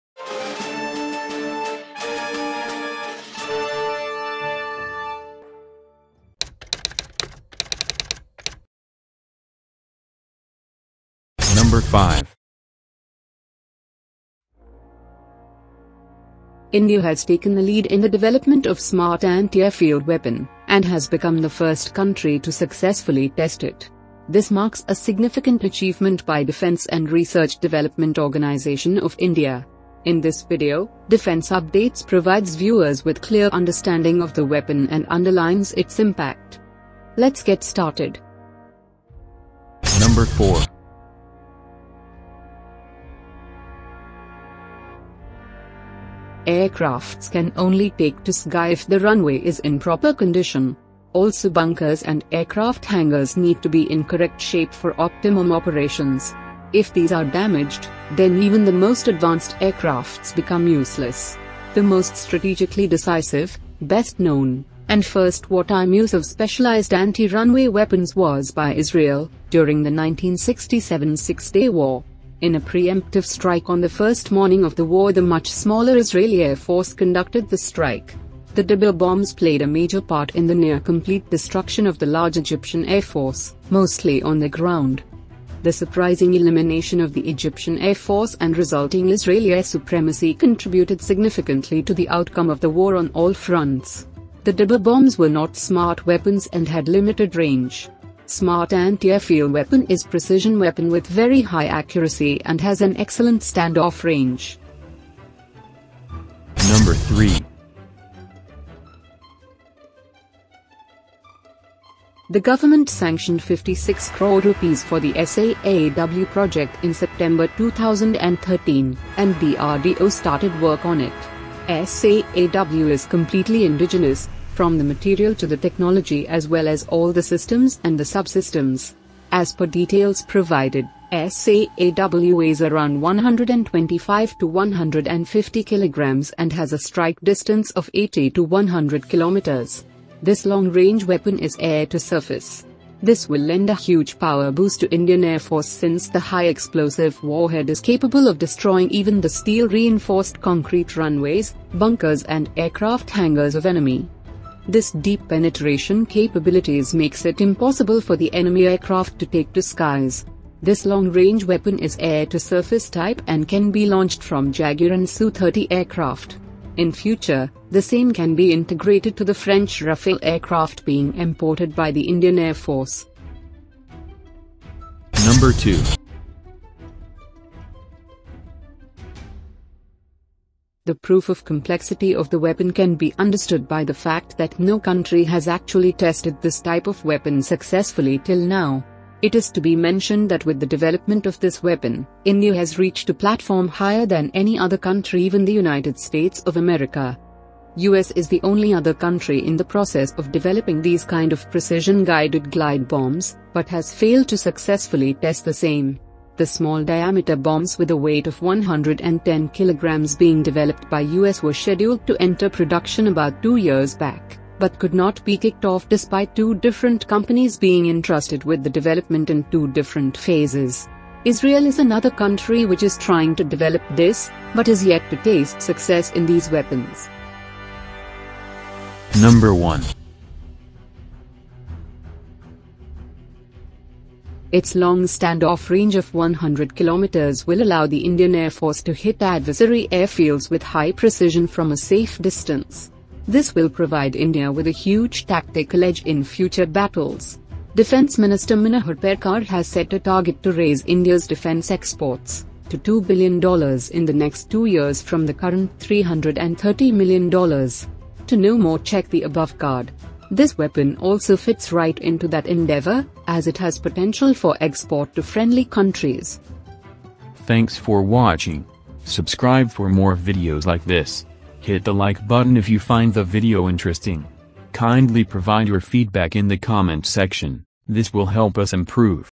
INDIA TESTS INDIGENOUS SMART ANTI-AIRFIELD WEAPON: TOP 5 FACTS
INTRODUCTION:

India has taken a lead in the development of Smart Anti-Airfield Weapon (SAAW), & has become the first country to successfully test it.
This marks a significant achievement by Defense and Research Development Organization (DRDO) of India.

In this video, Defense Updates provides viewers with clear understanding of the weapon and underlines its impact.

BACKGROUND:

Aircrafts can only take to sky if the runway is in proper condition.  Also bunkers and aircraft hangers need to be in correct shape for optimum operations. If these are damaged, then even the most advanced aircrafts become useless.

The most strategically decisive, best known, and first wartime use of specialized anti-runway weapons was by Israel, during the 1967 Six-Day War.
In a preemptive strike on the first morning of the war the much smaller Israeli Air Force conducted the strike.

The dibber bombs played a major part in the near complete destruction of the large Egyptian Air Force, mostly on the ground.  The surprising elimination of the Egyptian air force and resulting Israeli air supremacy contributed significantly to the outcome of the war on all fronts.

The dibber bombs were not smart weapons and had limited range.

Smart Anti-Airfield Weapon is precision weapon with very high accuracy and has an excellent standoff range.

TECHNICAL DETAILS:

The government sanctioned 56 crore rupees for the  S A A W project in September 2013, and DRDO started work on it.

S A A W is completely indigenous, from the material to the technology as well as all the systems and the sub-systems.
As per details provided, S A A W weighs around 125-150 kg and has a strike distance of 80-100 km. This long range weapon is Air to Surface.

This will lend a huge power boost to Indian Air Force (IAF) since the high explosive warhead is capable of destroying even the steel reinforced concrete runways, bunkers and aircraft hangers of enemy.

This deep penetration capabilities makes it impossible for the enemy aircraft to take to skies.

This long range weapon is Air to Surface type and can be launched from Jaguar and Su-30 aircraft. In future, the same can be integrated to the French Rafale aircraft being imported by the Indian Air Force.

COMPLEX TECHNOLOGY:

The proof of complexity of the weapon can be understood by the fact that no country has actually tested this type of weapon successfully till now.

It is to be mentioned that with the development of this weapon, India has reached a platform higher than any other country even the United States of America. US is the only other country in the process of developing these kind of precision-guided glide bombs, but has failed to successfully test the same.

The Small Diameter Bombs (SDB) with a weight of 110 kg being developed by US were scheduled to enter production about two years back, but could not be kicked off despite two different companies being entrusted with the development in two different phases.

Israel is another country which is trying to develop this, but is yet to taste success in these weapons.

CONCLUSION

Its long standoff range of 100 km will allow the Indian  Air Force to hit adversary airfields with high precision from a safe distance.

This will provide India with a huge tactical edge in future battles.
Defense Minister Manohar Parrikar has set a target to raise India’s defense exports to $2 billion in the next two years from the current  $330 million. To know more check the above card.
This weapon also fits right into that endeavor, as it has potential for export to friendly countries.